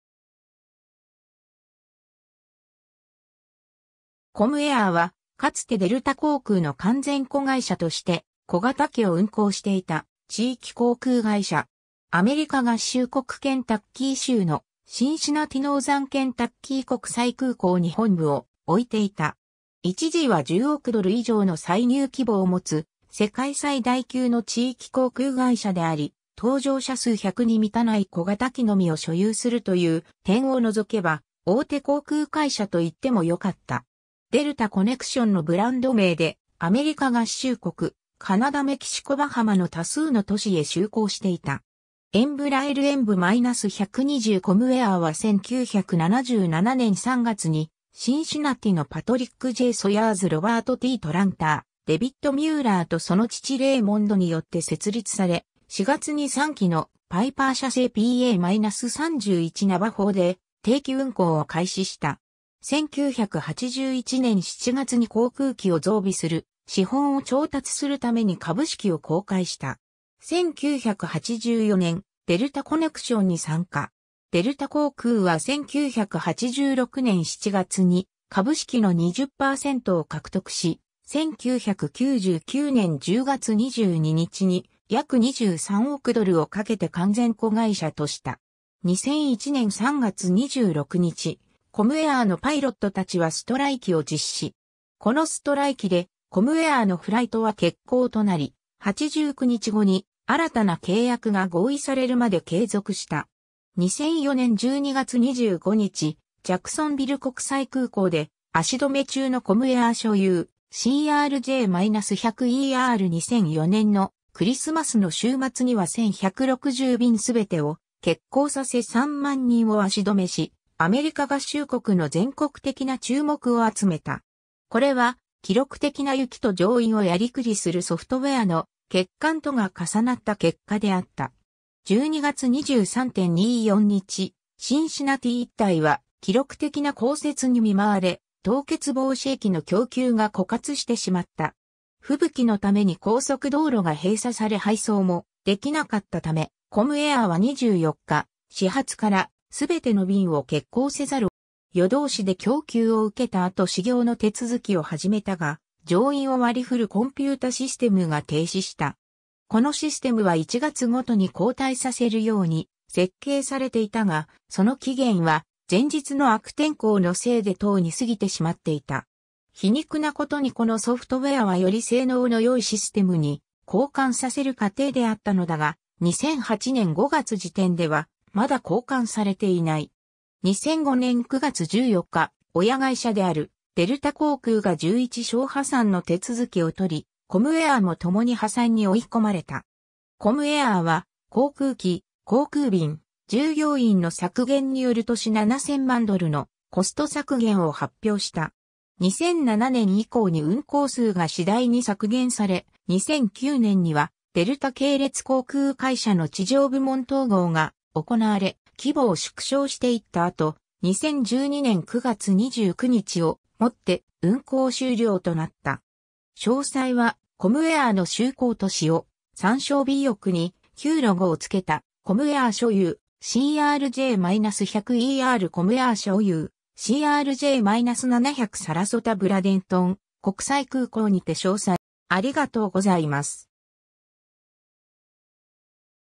コムエアー
コムエアーはかつてデルタ航空の完全子会社として小型機を運航していた地域航空会社。
アメリカ合衆国ケンタッキー州のシンシナティ・ノーザンケンタッキー国際空港に本部を置いていた。
一時は10億ドル以上の歳入規模を持つ世界最大級の地域航空会社であり、搭乗者数100に満たない小型機のみを所有するという点を除けば大手航空会社といってもよかった。
デルタ・コネクションのブランド名で、アメリカ合衆国・カナダ・メキシコ・バハマの多数の都市へ就航していた。
 エンブラエル EMB-120 コムエアーは1977年3月にシンシナティのPatrick J. Sowers・Robert T. Tranter・David Muellerとその父Raymondによって設立され、4月に3機のパイパー社製PA-31ナバホで定期運航を開始した。
1981年7月に航空機を増備する資本を調達するために株式を公開した。
1984年デルタ・コネクションに参加。
デルタ航空は1986年7月に株式の20%を獲得し、1999年10月22日に約23億ドルをかけて完全子会社とした。
 2001年3月26日、コムエアーのパイロットたちはストライキを実施。
このストライキでコムエアーのフライトは欠航となり、89日後に新たな契約が合意されるまで継続した。
 2004年12月25日ジャクソンビル国際空港で足止め中のコムエアー所有 CRJ-100ER 2004年のクリスマスの週末（25・26日）には1160便全てを欠航させ3万人を足止めし、アメリカ合衆国の全国的な注目を集めた。
これは記録的な雪と乗員をやりくりするソフトウェアの欠陥とが重なった結果であった。
12月23・24日、シンシナティ一帯は記録的な降雪に見舞われ、凍結防止液の供給が枯渇してしまった。
吹雪のために高速道路が閉鎖され配送もできなかったため、コムエアーは24日始発から全ての便を欠航せざるを得なかった。
夜通しで供給を受けた後始業の手続きを始めたが、乗員を割り振るコンピュータシステムが停止した。
このシステムは1月ごとに交替させるように設計されていたが、その期限は前日の悪天候のせいでとうに過ぎてしまっていた。
皮肉なことにこのソフトウェアはより性能の良いシステムに交換させる過程であったのだが、2008年5月時点ではまだ交換されていない。
 2005年9月14日、親会社であるデルタ航空が11章破産の手続きを取り、コムエアーも共に破産に追い込まれた。
コムエアーは航空機・航空便・従業員の削減による年7000万ドルのコスト削減を発表した。
2007年以降に運航数が次第に削減され、2009年にはデルタ系列航空会社の地上部門統合が行われ、規模を縮小していった後、2012年9月29日をもって運航終了となった。
 詳細は「コムエアーの就航都市」を参照 尾翼に旧ロゴをつけたコムエアー所有 CRJ-100ER（オクラホマ国際空港にて） コムエアー所有 CRJ-700 サラソタ・ブラデントン国際空港にて 詳細は「コムエアー3272便墜落事故」を参照 詳細は「コムエアー5191便離陸失敗事故」を参照